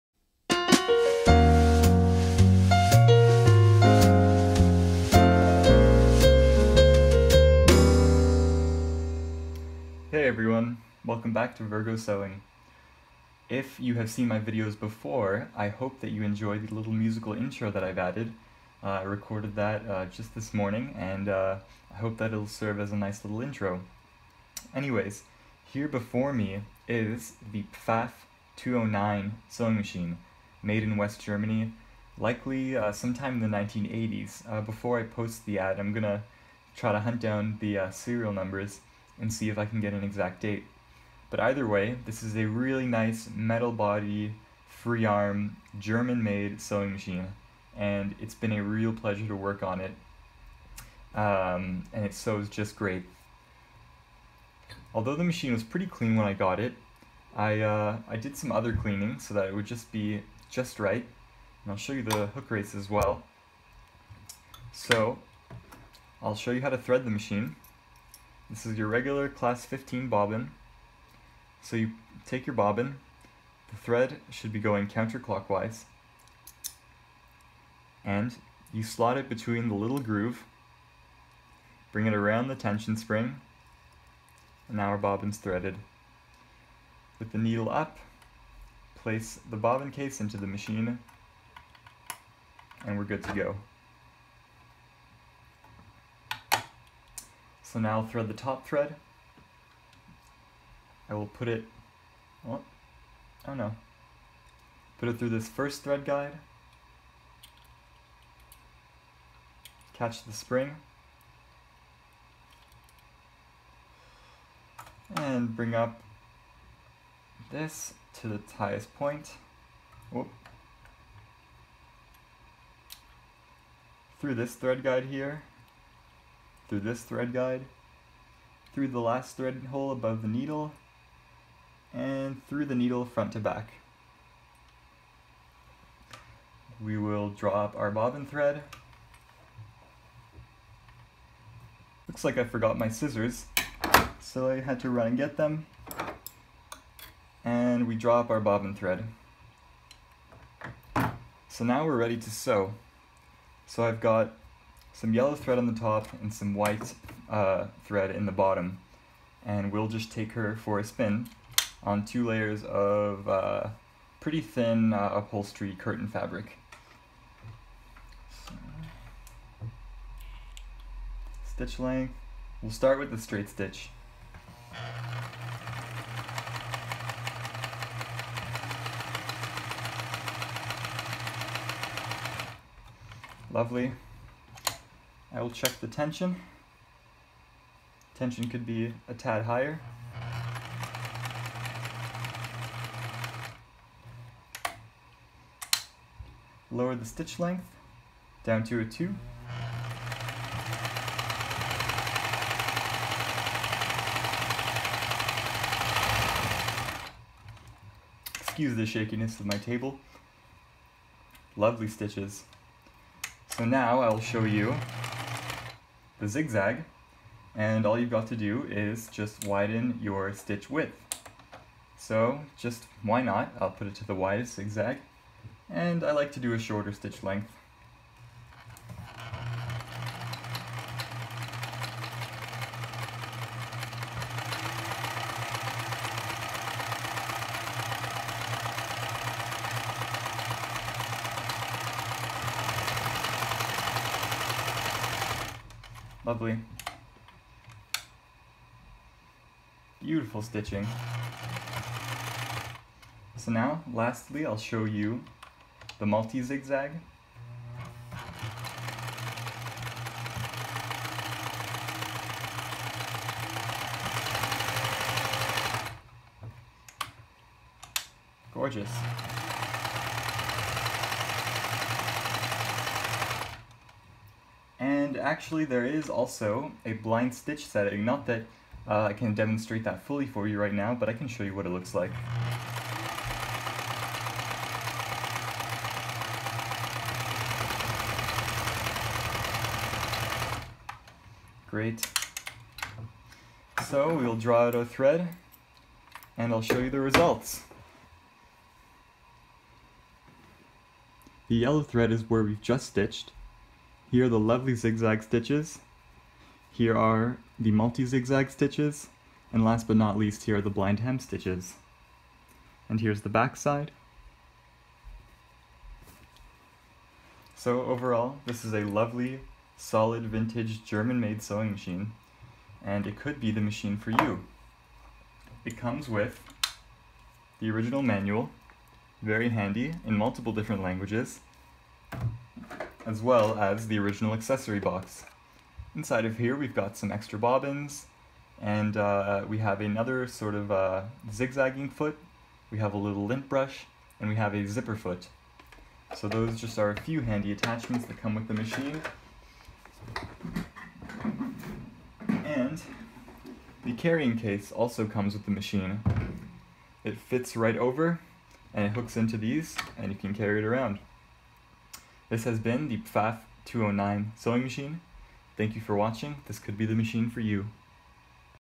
Pfaff 209 Free-Arm Sewing Machine - Serviced! (threading + demonstration)
One thing I forgot to demonstrate was the reverse feed. Fear not! The reverse works perfectly.